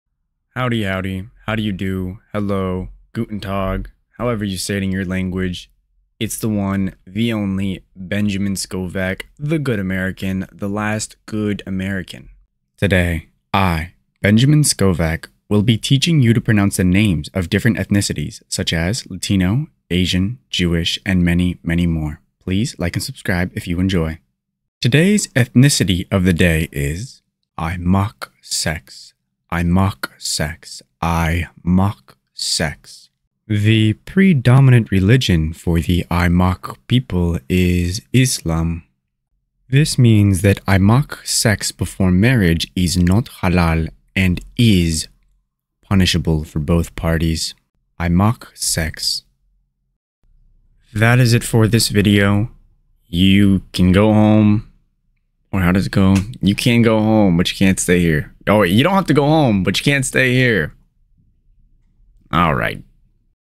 How to pronounce Aimaq sex?(CORRRECTLY)
Welcome!
Throughout the entirety of my channel, you will learn to pronounce vocabulary the correct way. In today’s video, you will learn to pronounce Aimaq sex the correct way.
Timestamps below:
0:00 intro
0:27 word of the day
0:37 examples of word
0:51 ending